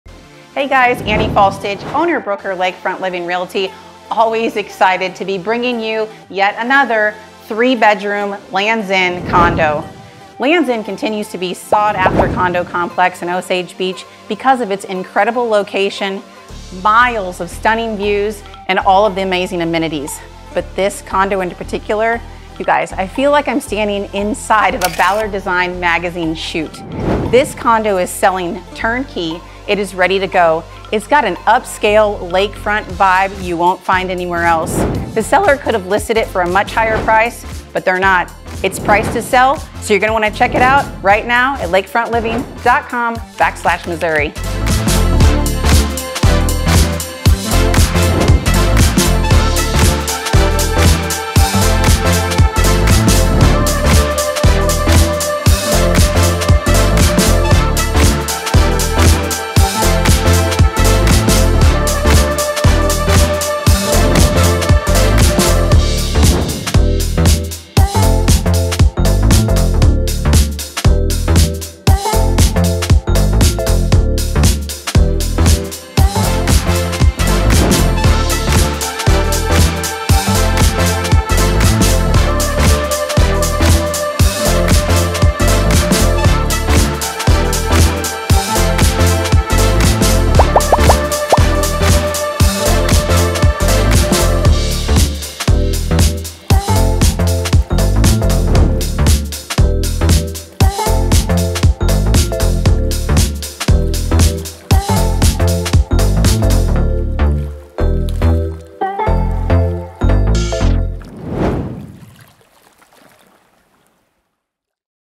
This Unit Is NEXT LEVEL - Unbelievable Lakefront Living Awaits!
Video Cred @studiovhype

When you envision a place to get a way form it all, THIS IS IT! Straight from a Ballard Design or Pottery Barn catalog this condo has the luxury finishes paired with soft and relaxing vibe. The combination of upgraded fixtures, furnishings and texture set this condo apart from the others. Just one flight from parking and steps from the elevator it has ease of access to parking and to the dock. elevator takes you all the way to dock level with in view of your unit. Screen deck gives MILES of 180 degree lake views that NEVER get old. Lands End complex amenities and location are second to none. Full clubhouse with a huge private gym and pool table is the perfect place for family to gather on occasions or rainy days. Lazy River, kids water play area, playground, lakefront, zero entry pool and security make this the home or home away from home you have been waiting for. 14X40 boat slip is the PERFECT sought after size and included in the price along with everything you see in the condo.